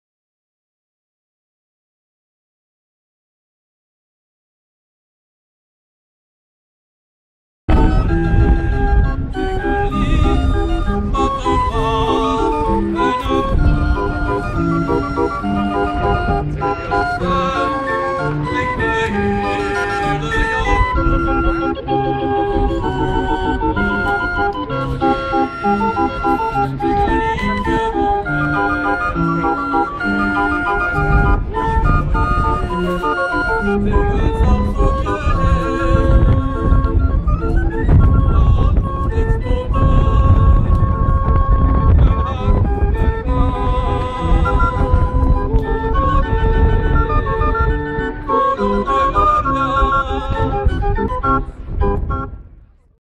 Barrel Organ Street Performance @ Antwerpen, Belgium
The barrel organ was the traditional instrument of organ grinders.
The term hurdy gurdy is sometimes mistakenly applied to a small,
portable barrel organ that was frequently played by organ grinders and buskers.
A barrel organ (or roller organ) is a mechanical musical
instrument consisting of bellows and one or more ranks
of pipes housed in a case, usually of wood, and often
highly decorated. The basic principle is the same as a
traditional pipe organ, but rather than being played by
an organist, the barrel organ is activated either by a person
turning a crank, or by clockwork driven by weights or springs.
The pieces of music are encoded onto wooden barrels
(or cylinders), which are analogous to the keyboard
of the traditional pipe organ.
The size of the barrel will depend on the number of notes
in the organ and the length of the tune to be played.
The more notes, the longer the barrel.
The longer the tune, the greater the diameter.